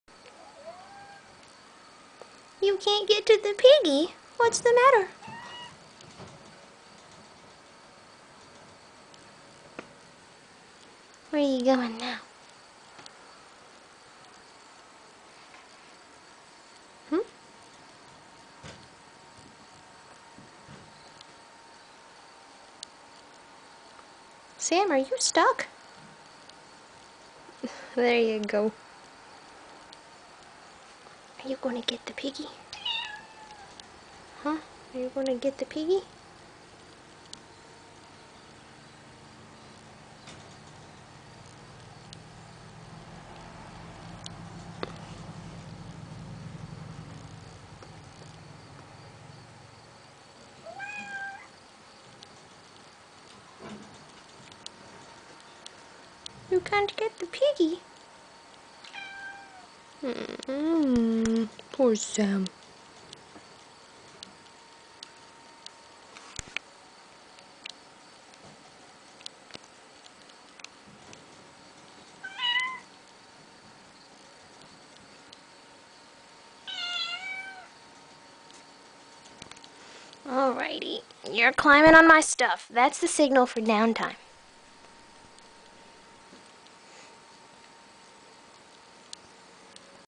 Guinea's cage - Take 2
Sam's best (non-human) friend is Guinea, my sister's guinea pig. Guinea is about as big as Sam... and she hates the furry bugger. Sam never leaves Guinea alone, and it annoys the crap out of the pig. Sam thinks it's hilarious. Guinea wants to kill her.